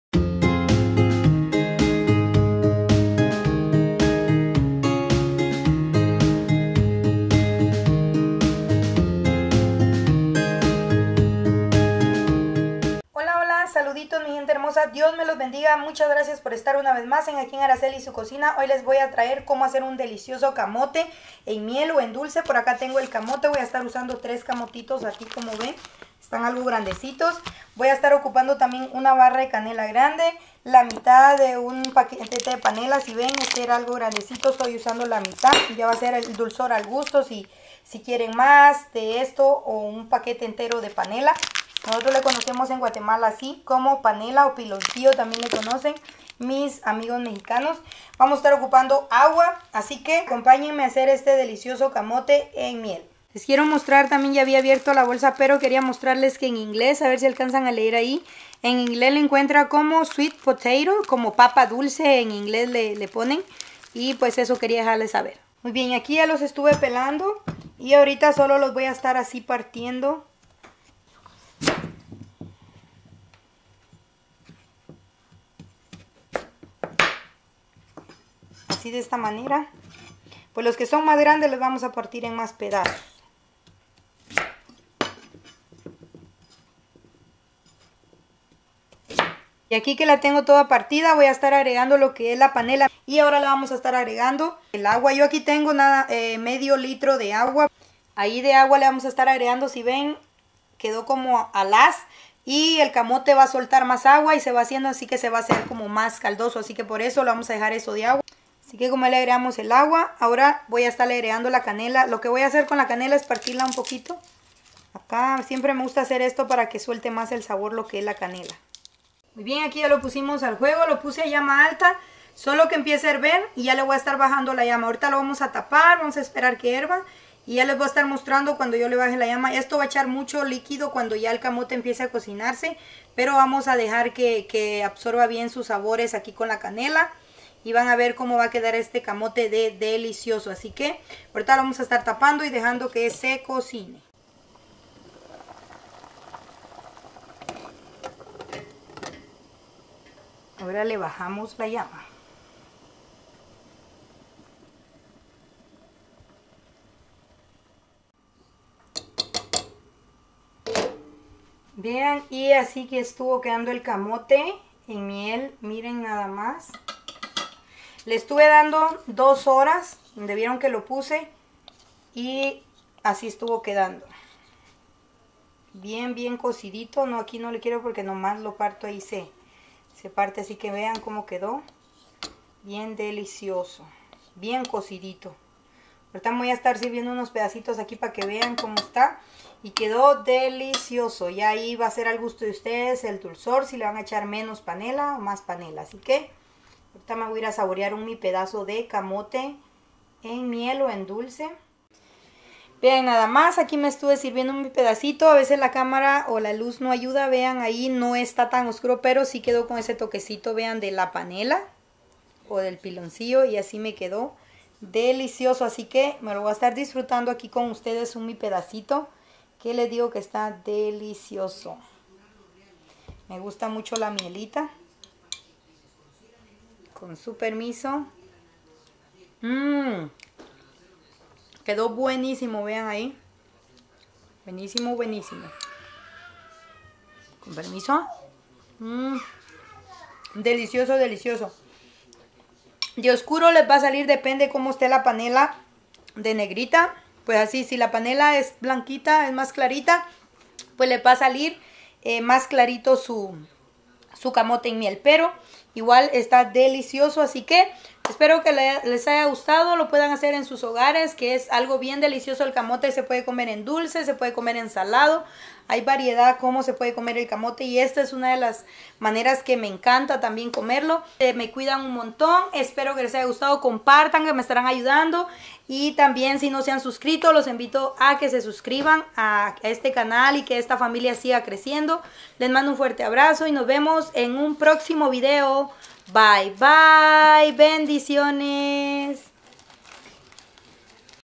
Camote en miel
Hola saludos., Dios me los bendiga aqui les comparto esta rica receta de camote en miel que sale bien delicioso., espro les guste y me regalen ese hermoso like y se suscriban si aun no lo an hecho., se me cuidan un monton y nos vemos en otro video byeeee...